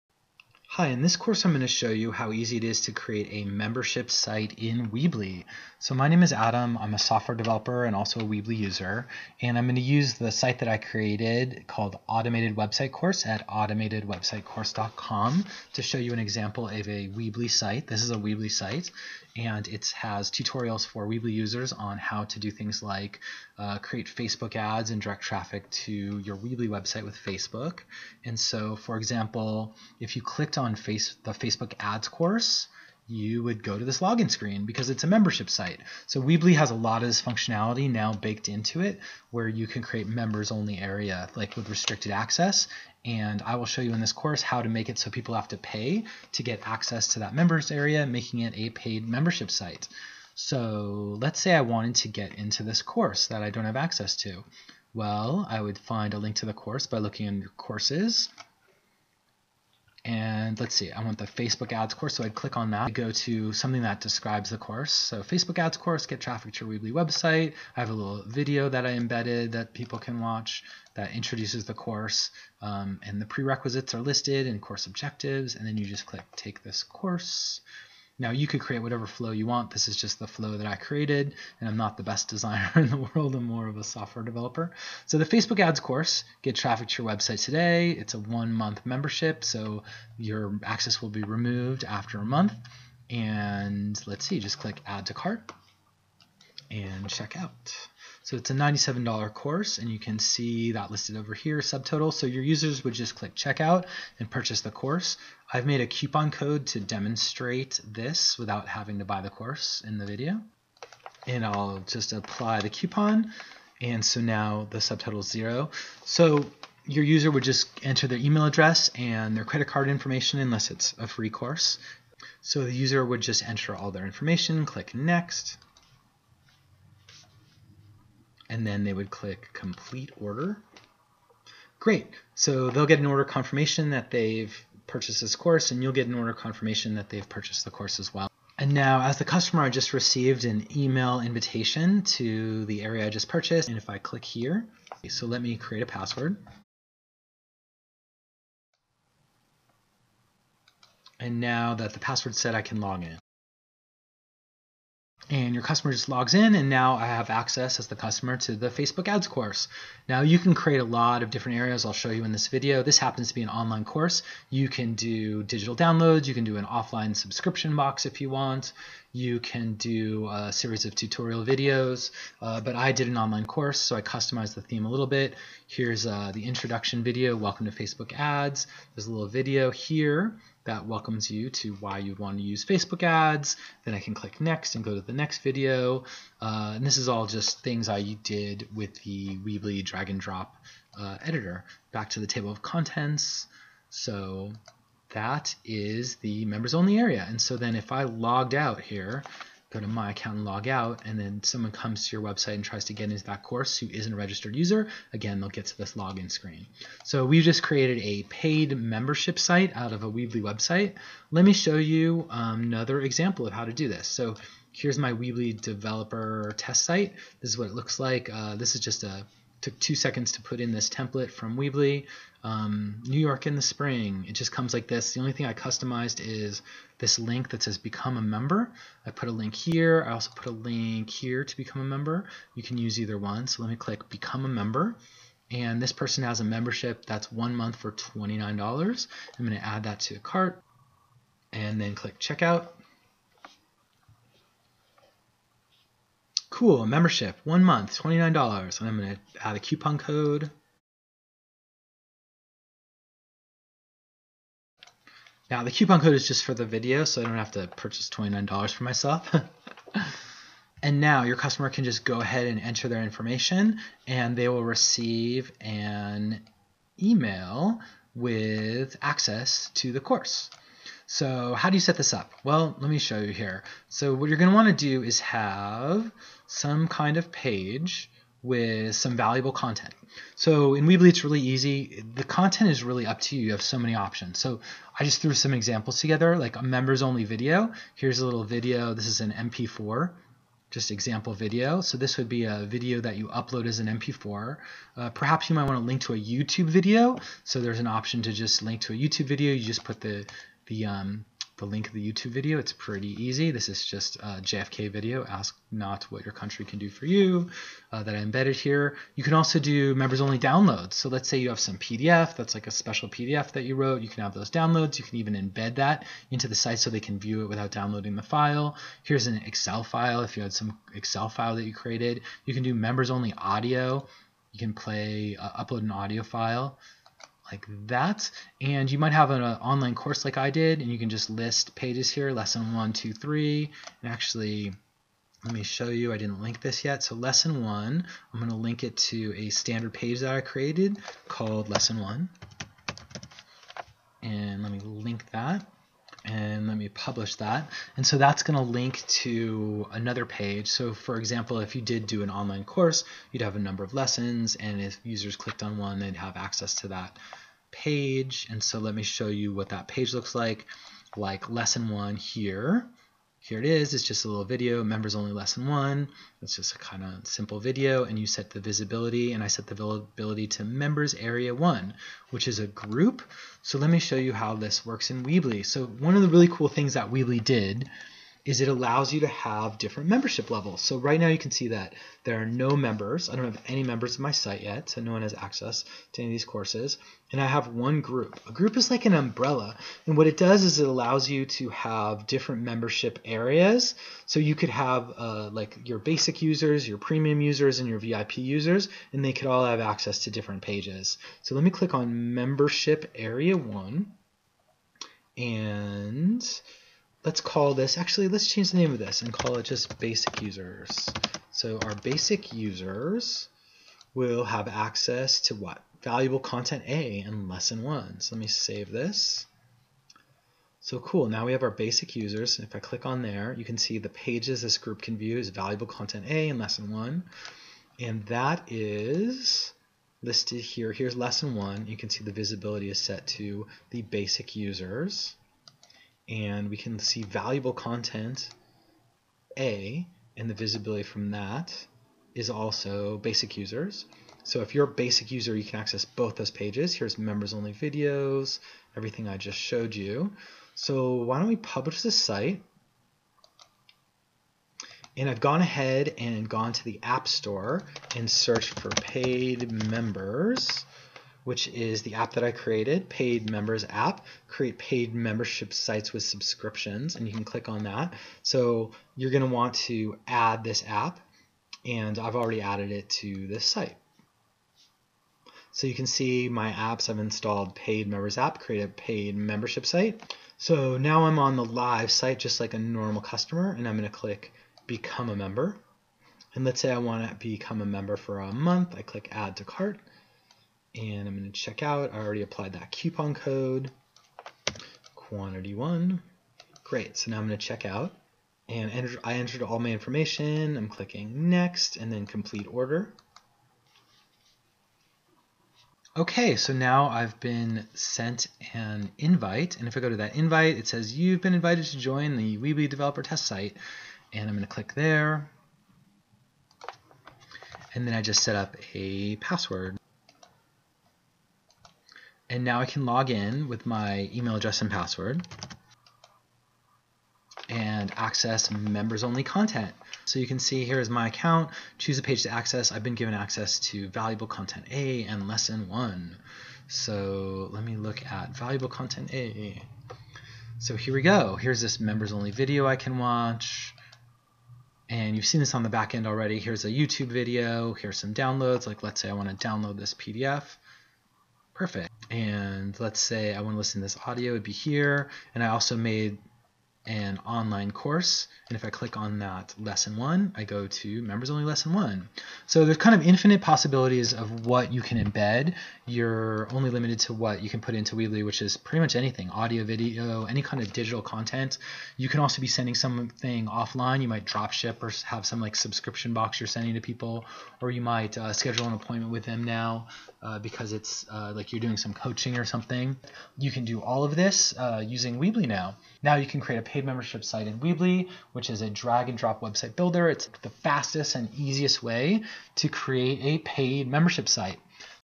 How to Make a Membership Site in Weebly
This course shows how to make a paid membership site using Weebly. It provides an example of a live, production membership site created with Weebly and Paid Members App which is available in the Weebly app store.